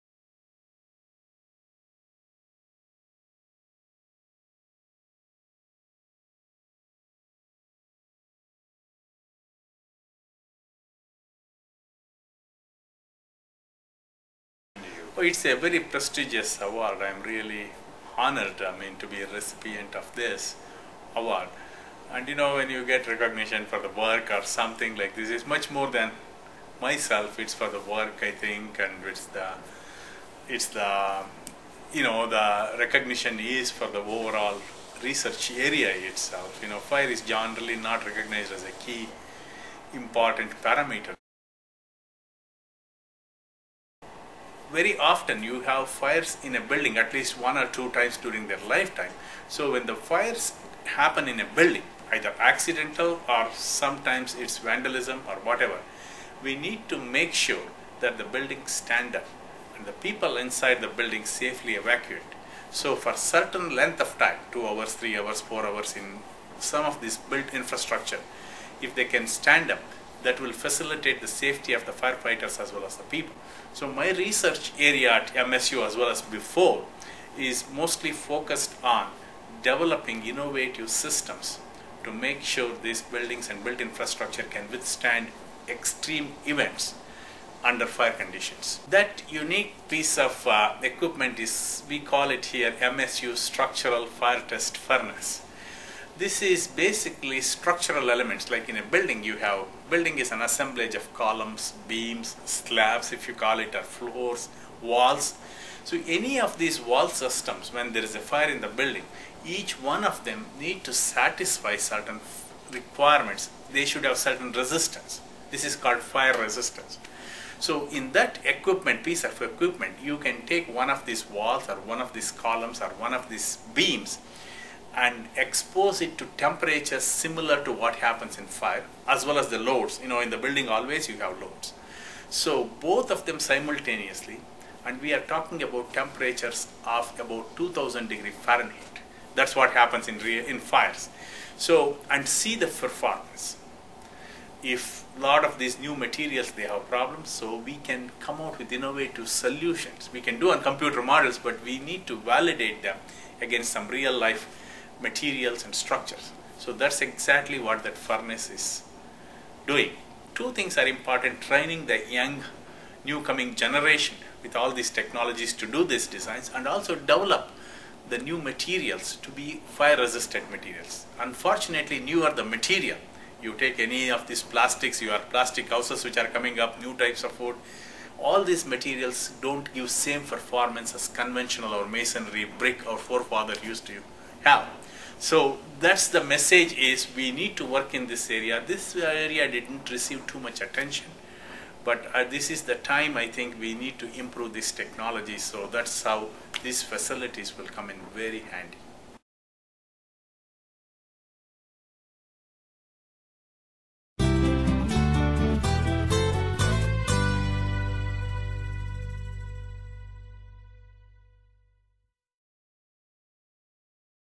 Faculty conversations: Venkatesh Kodur
Venkatesh Kodur, civil and environmental engineering professor, talks about his work leading Michigan State University's Structural Fire Testing Facility. Kodur was recently named a fellow of the Canadian Academy of Engineering.